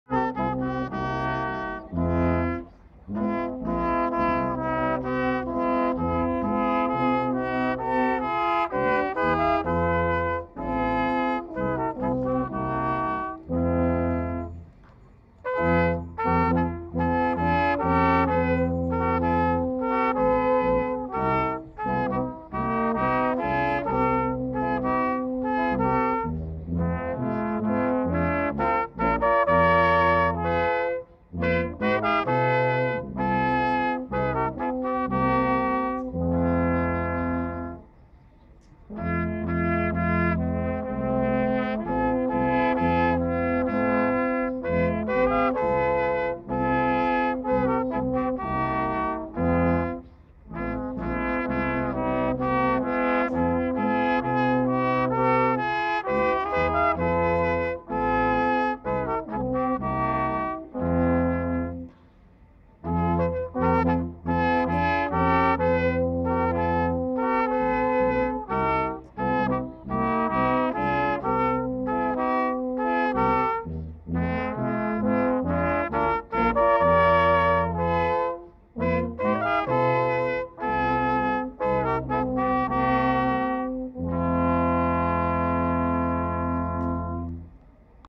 Waldweihnacht_02 © OlmHERZ
Reichenstoa Blos bei Waldweihnacht in Bruck a.d. Mur, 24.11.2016_02 © OlmHERZ